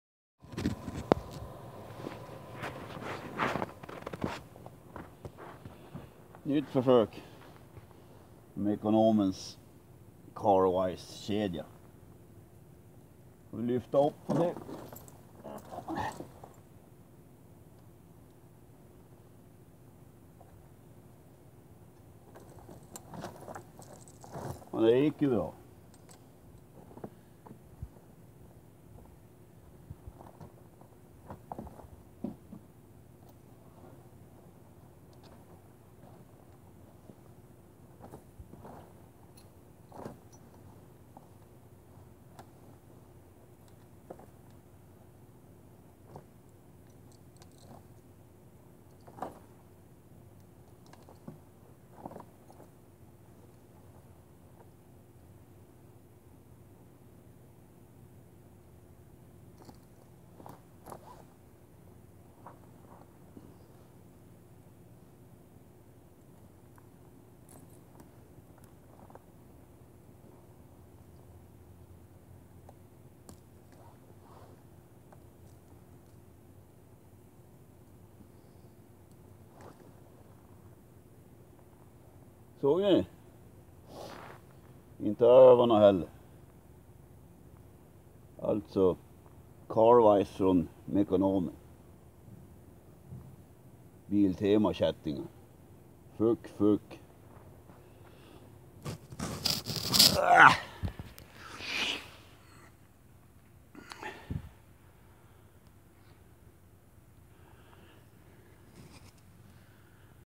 Mekonomens fantastiska snökedjor på Volvo V70
Med Carwise 9 mm snökedja var det enkelt att montera på första försöket. Med Biltemas gick det inte alls fast jag höll på flera dar. Beror antagligen på att denna kätting är tunnare i alla delar, även vajern som ska sitta på insidan hjulet.